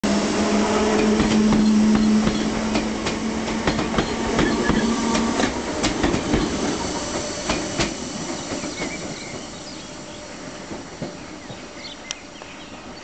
508207 departing Tonbridge
Connex liveried 508207 departs Tonbridge with a SouthEastern service for London Bridge on 5/6/2007.